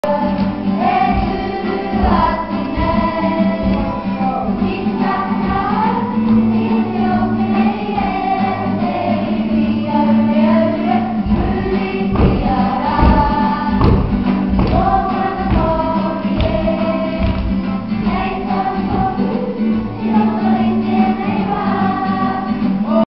Te Kopuru Kapa Haka
Te Kopuru Kapa Haka perform at Retha's farewell December 2011